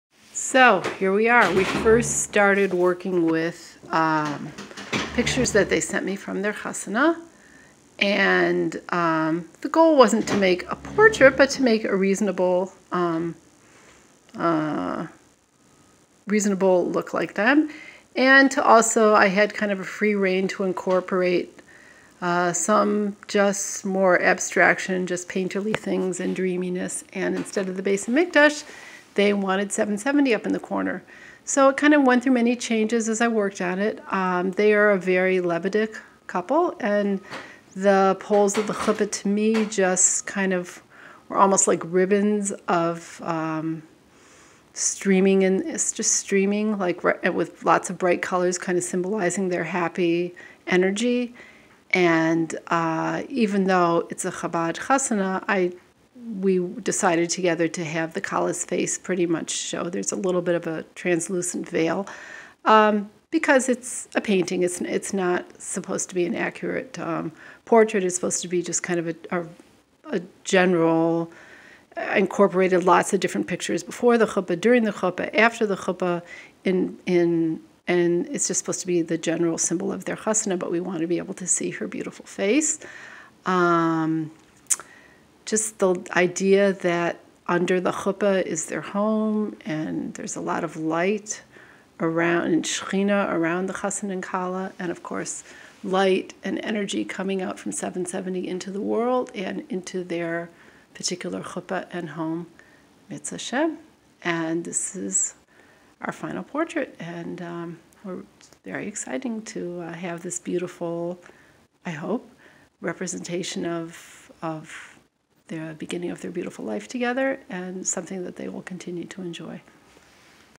Miriam Karp Artist At The Leviim Judaica Art Gallery
Miriam Karp of the Leviim Judaica Art Gallery describing the process how she paints.